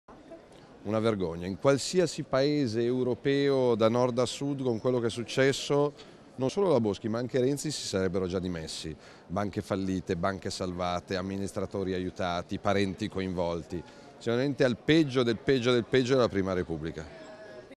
SALVINI: CASO BOSCHI BANCA ETRURIA E' PEGGIO DEL PEGGIO DELLA PRIMA REPUBBLICA
(Agenzia VISTA) - Strasburgo, 15 Dicembre 2015 - Il leader della Lega Nord Salvini presente al Parlamento Europeo in seduta plenaria.